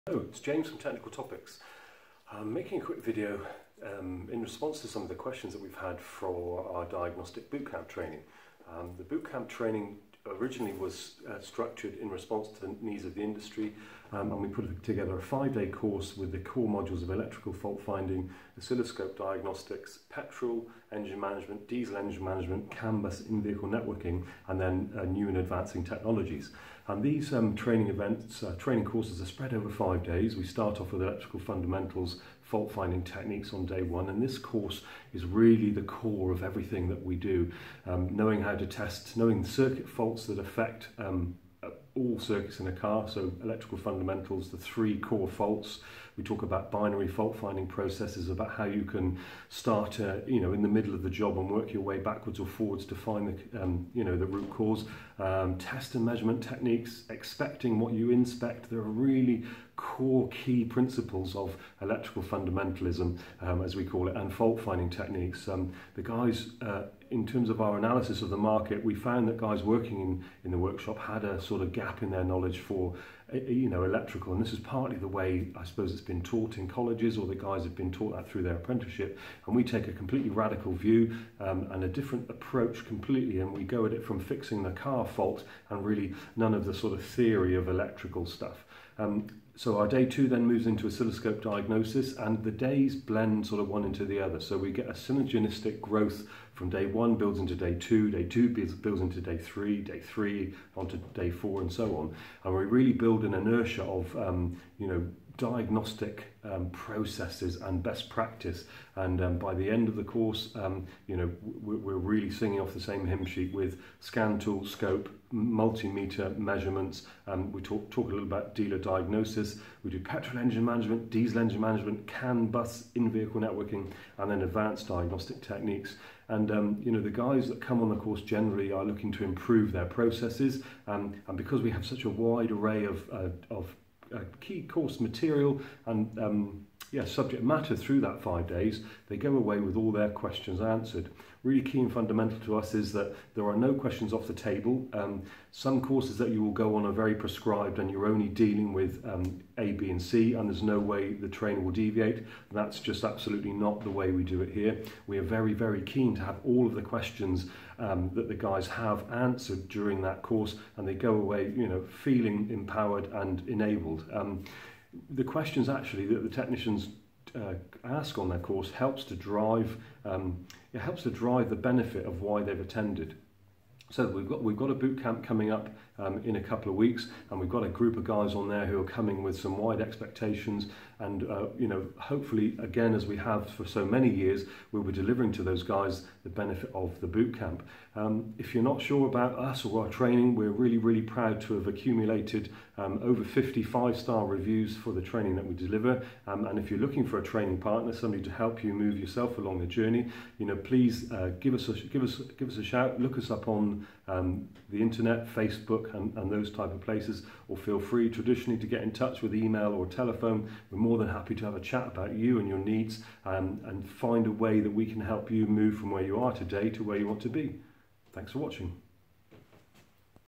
Why Diagnostic Bootcamp Training
Master Technician James Dillon talks about Tech Topics Diagnostic Bootcamp Training Course.